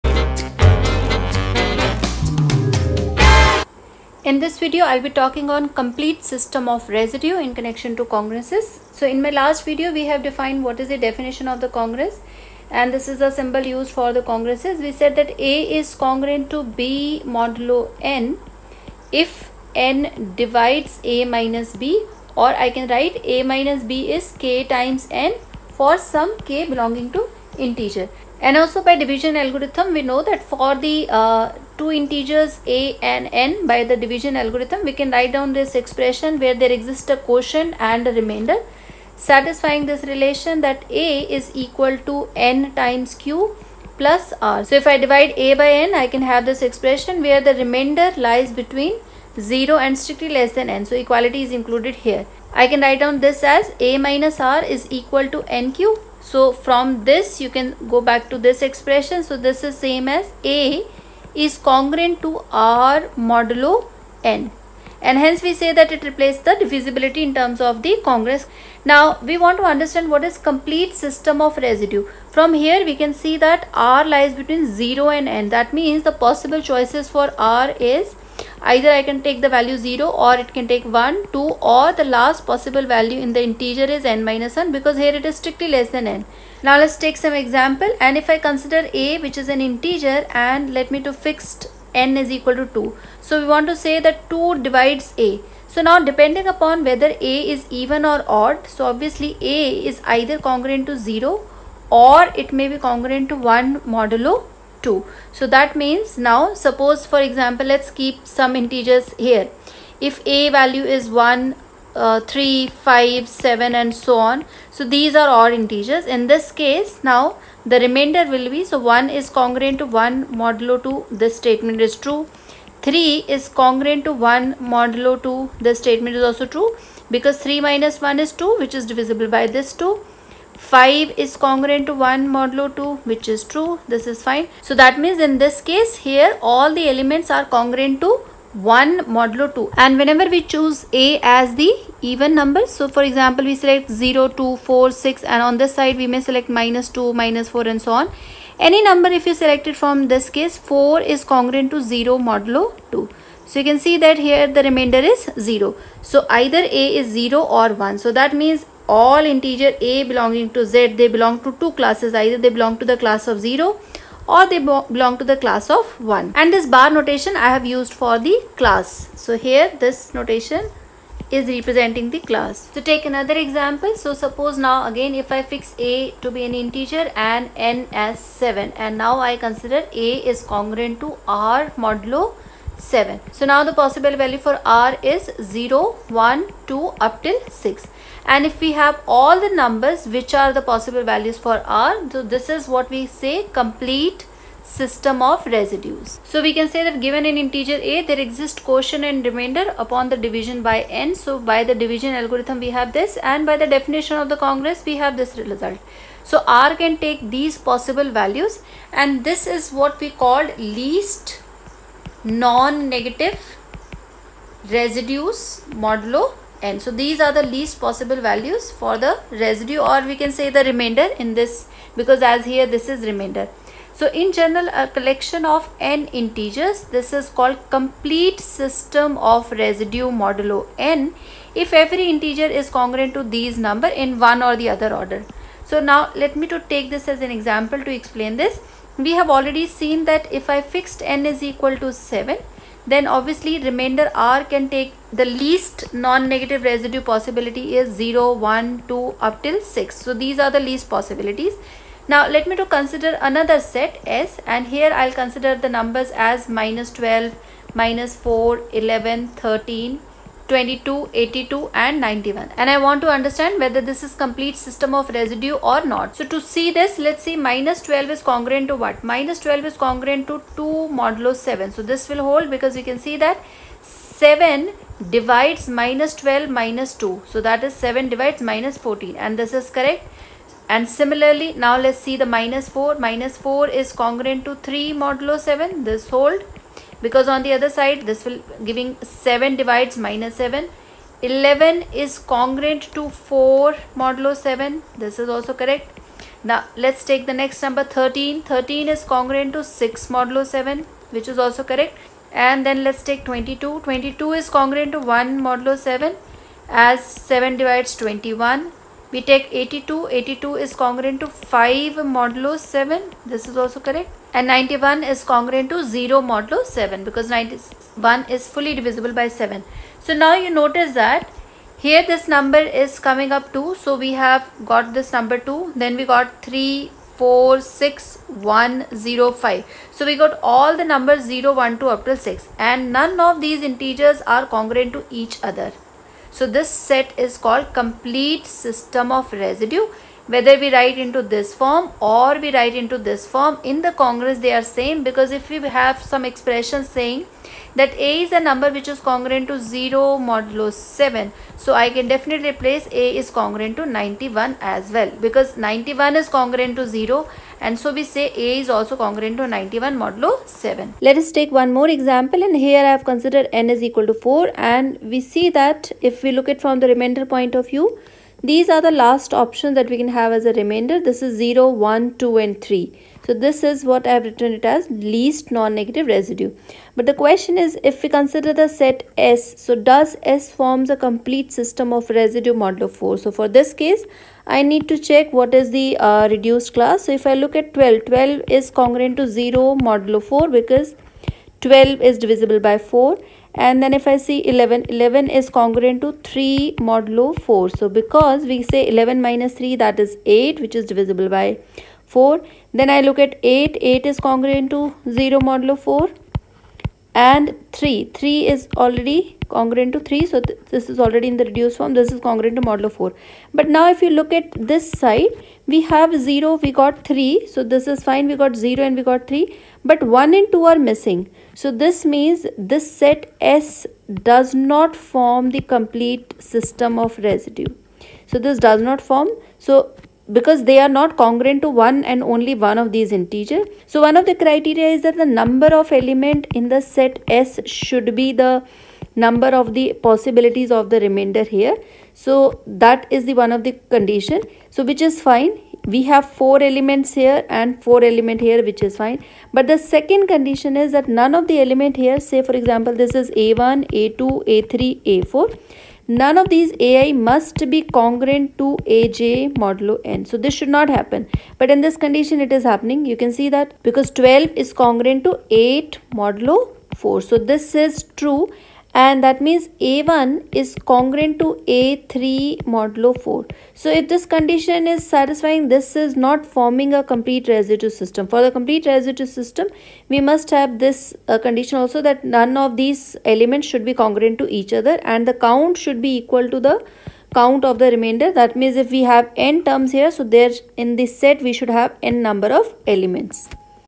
Congruences |Part 2| Complete System of Residues
This video is about Complete System of Residues in congruence